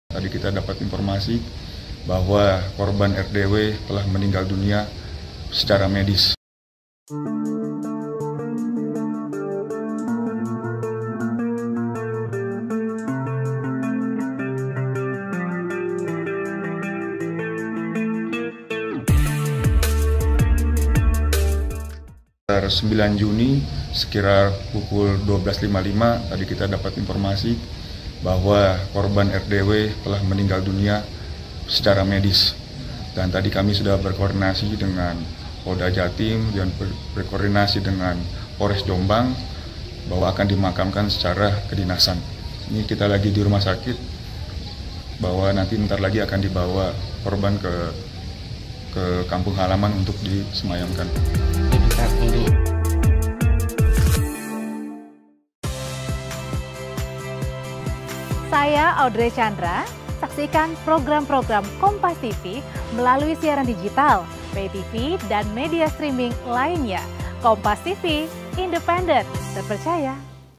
Polisi yang Dib4kar Istri di Mojokerto Akhirnya Meninggal Dunia
KOMPAS.TV-Polisi yang Dib4kar Istri di Mojokerto Akhirnya Meninggal Dunia